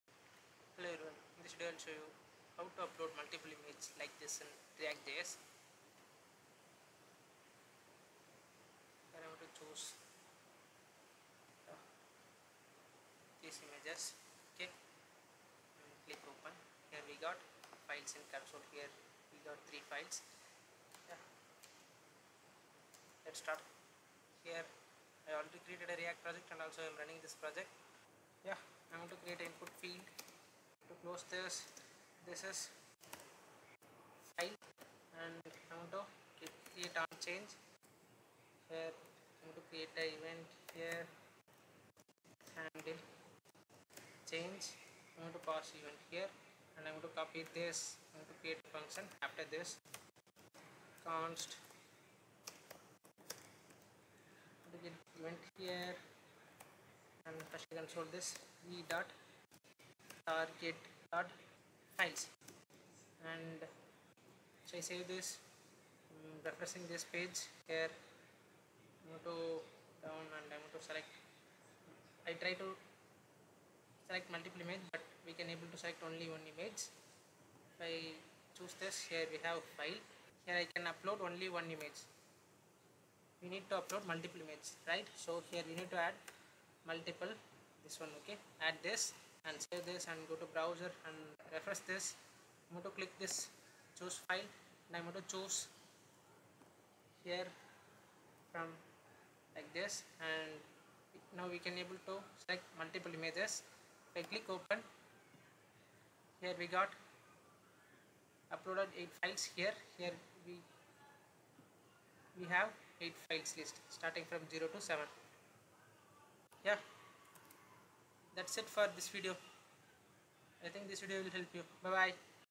How to Upload Multiple Images(Files) in React js
In this video, I have explained how to to upload multiple images in React.js, you need to create a form that allows users to select multiple files, handle the file input change event to capture the selected images, store these files in the component's state, and then implement a function to handle the form submission, which sends the images to the server or the desired destination.